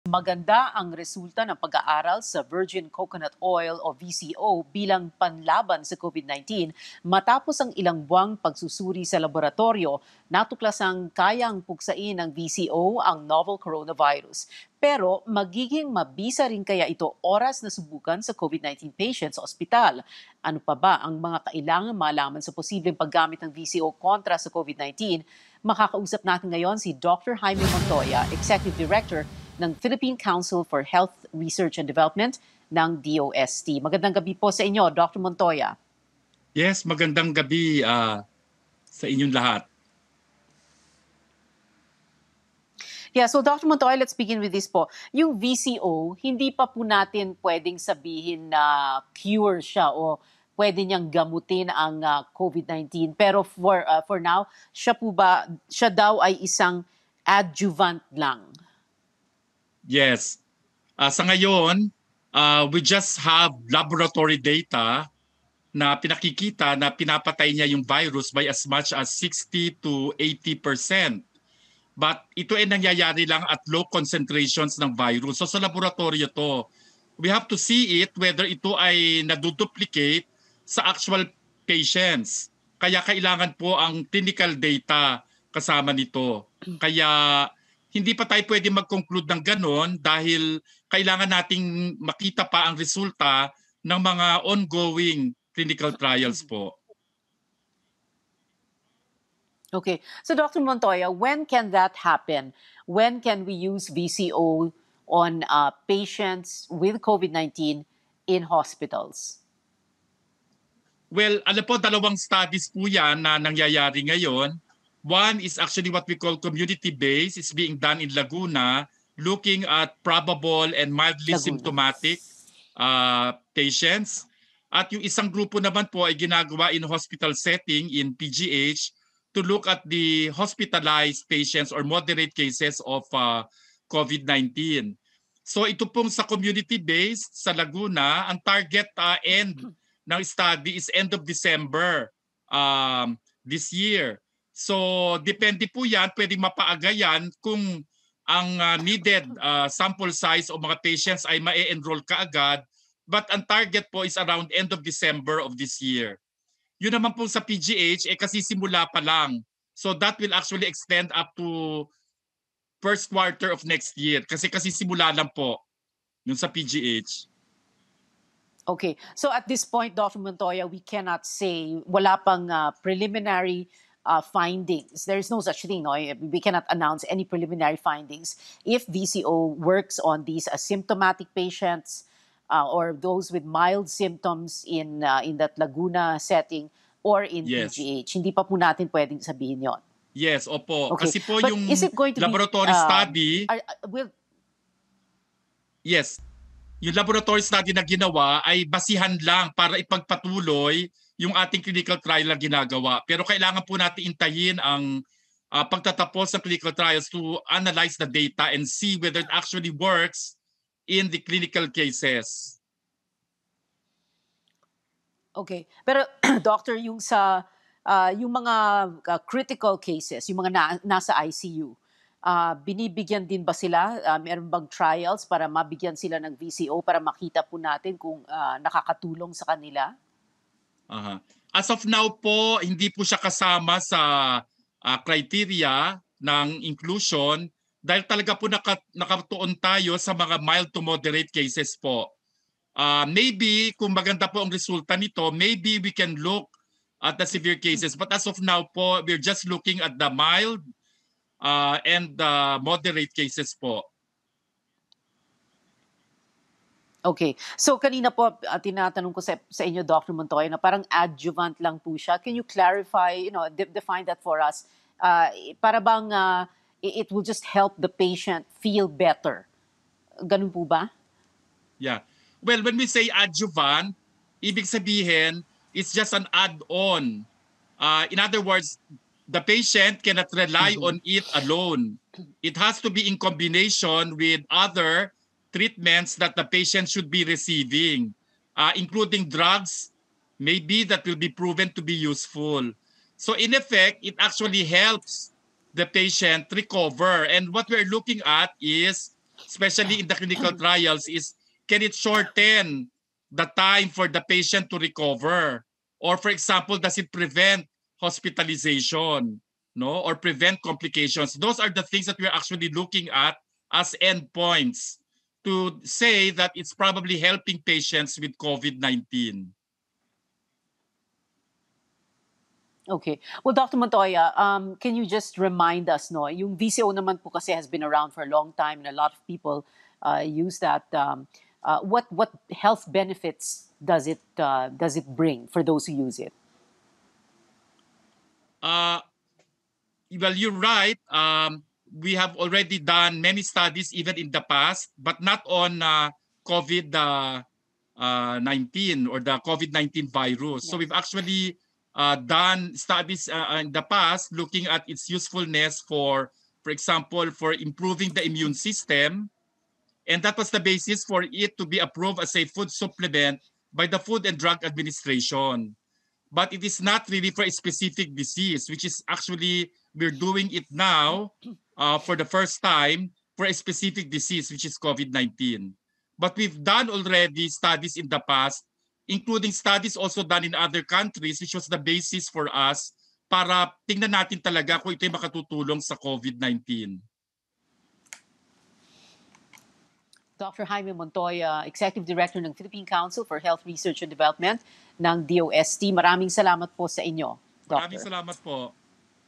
VCO kayang puksain ang virus sa laboratoryo
Ang potensyal ng virgin coconut oil laban sa COVID-19, tatalakayin natin kasama si DOST-PCHRD Executive Director Dr. Jaime Montoya.